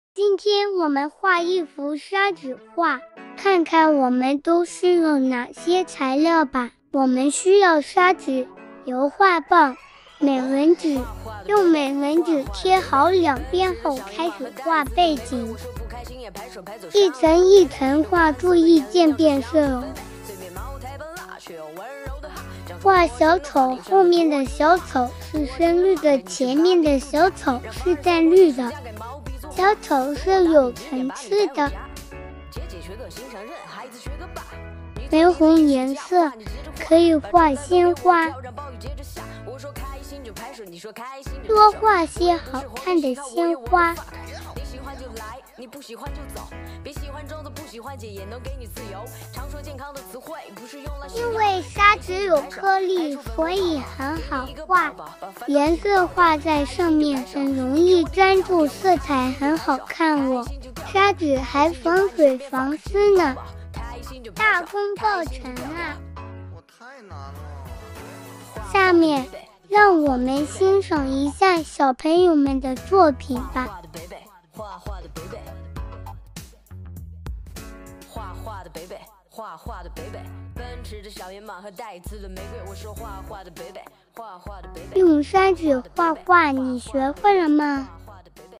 GRISH Creative Products--Sandpaper
Hello, dear parents and friends, have you been asked those questions as follows:
What is sandpaper painting?
How to draw a sandpaper painting?
What brushes to use?
What tips can you show the beauty of sandpaper painting?

Recently we have recorded a special video of sandpaper painting, hoping it can help in your situation．